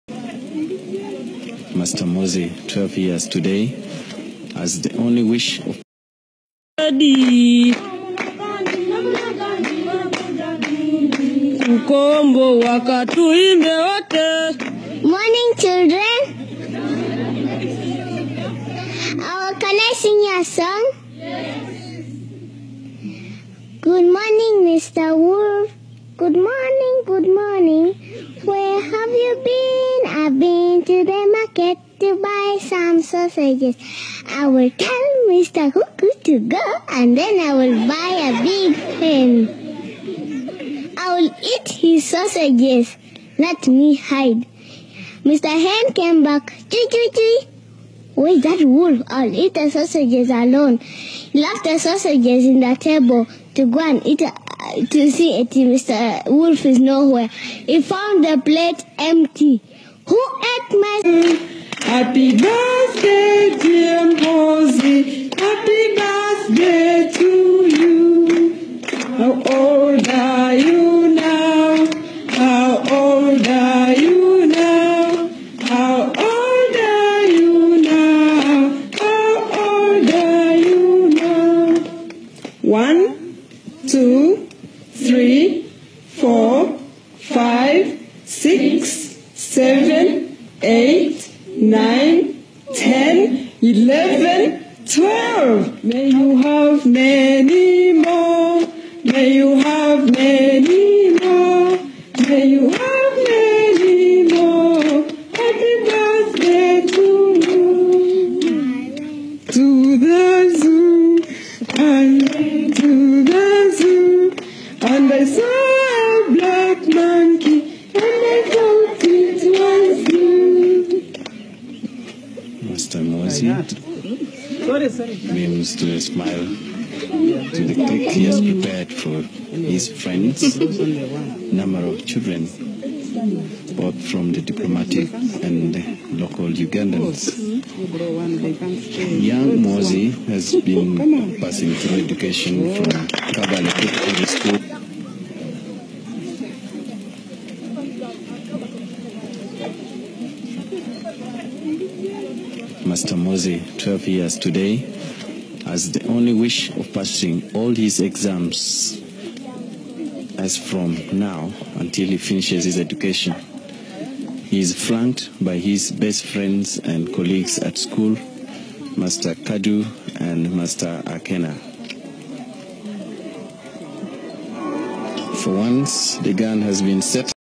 Rare video shows Muhoozi celebrating 12th birthday in 1986 after Museveni had taken power
Rare footage shows Muhoozi Kainerugaba celebrating his 12th birthday in 1986